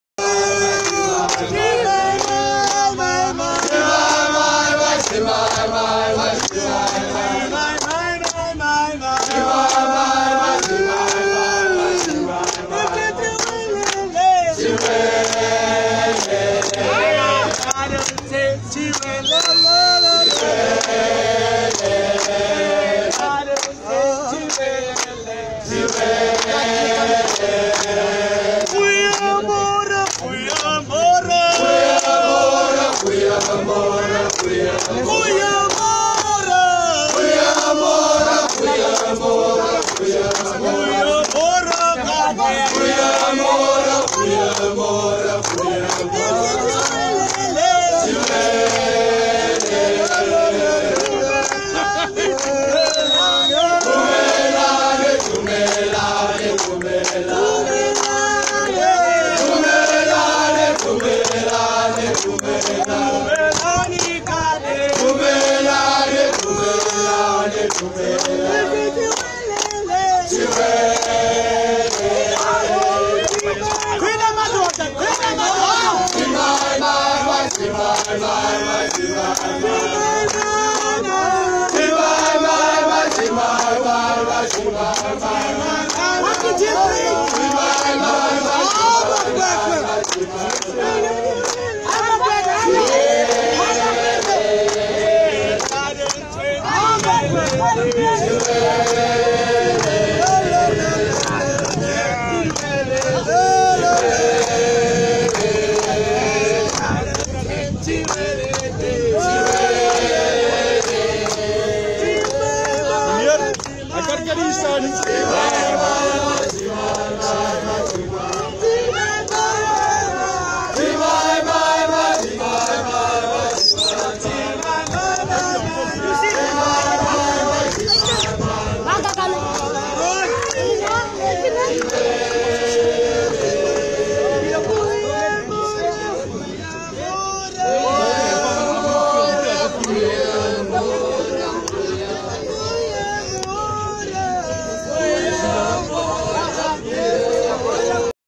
05 December 2020
Passionate Gardens supporters singing